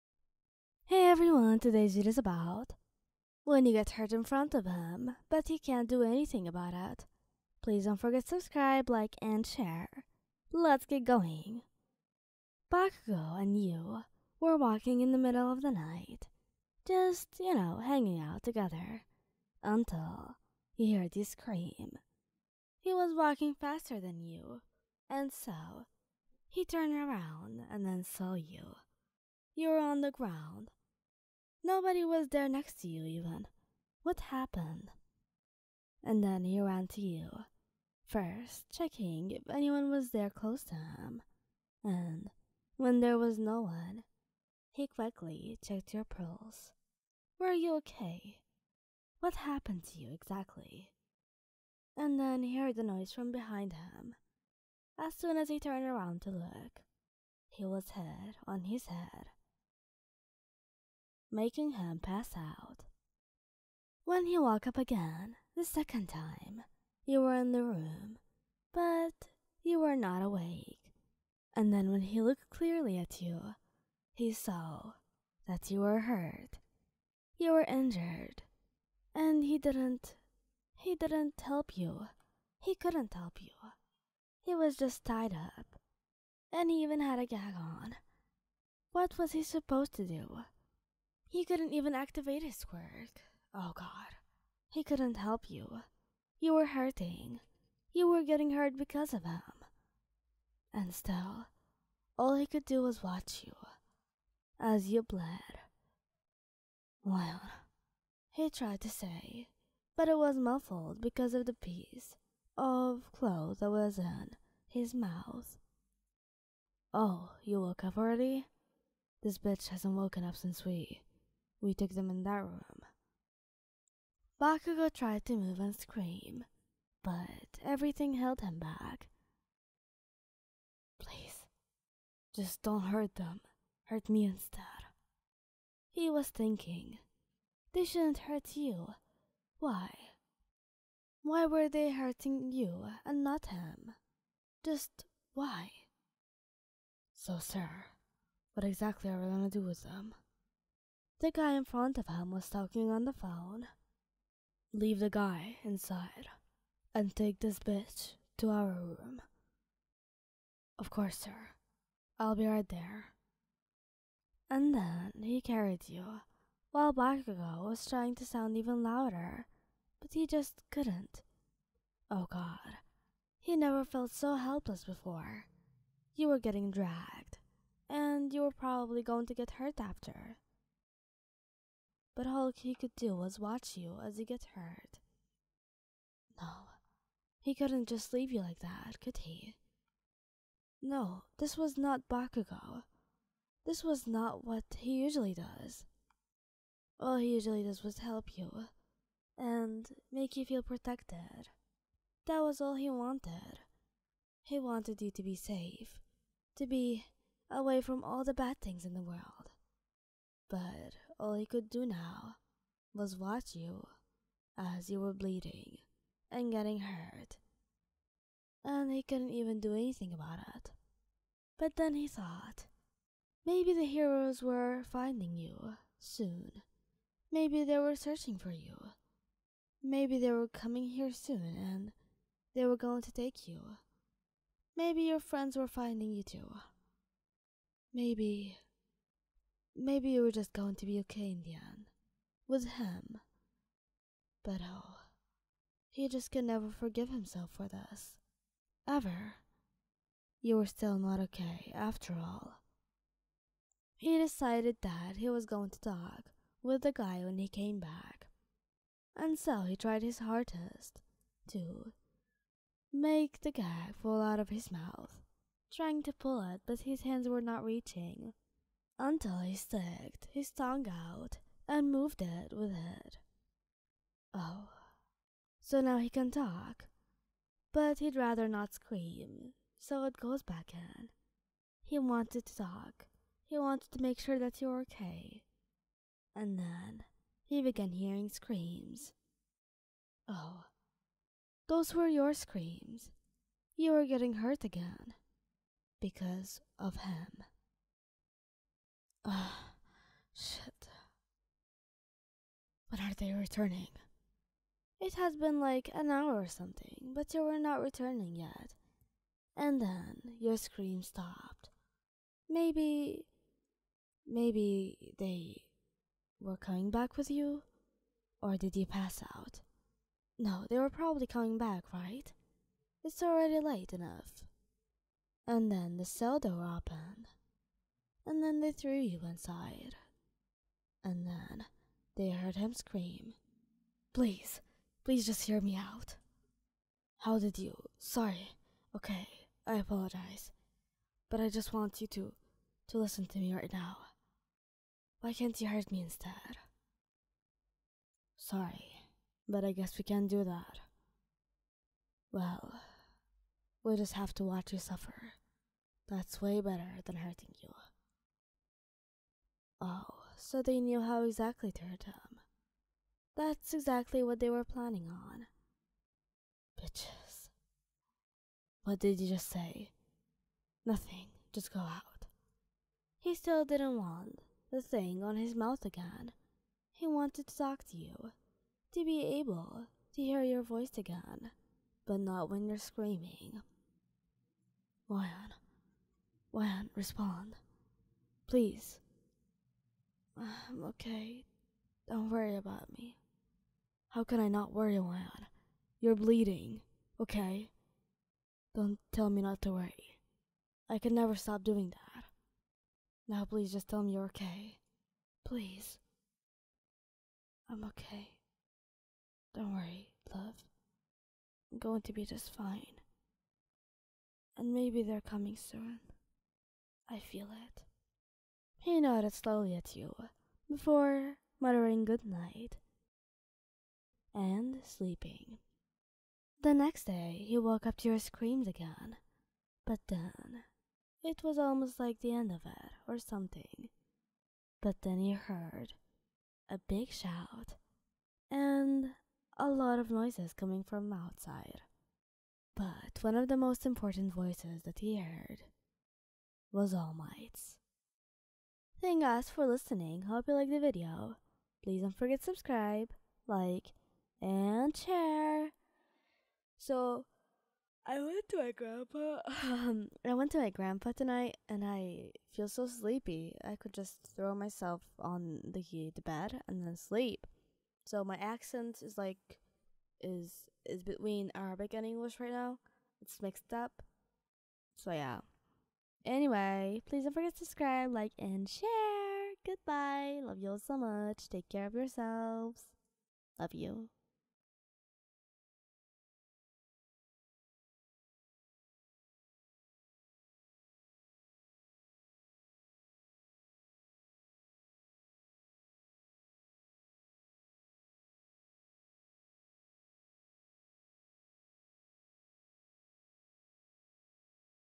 You get hurt in front of him but he can't do anything - Bakugou x Listener
Hii! Story is written by me (:

You get hurt in front of him but he can't do anything - Bakugou Katsuki x Listener

Thank you guys for listening and reading!!

Gn!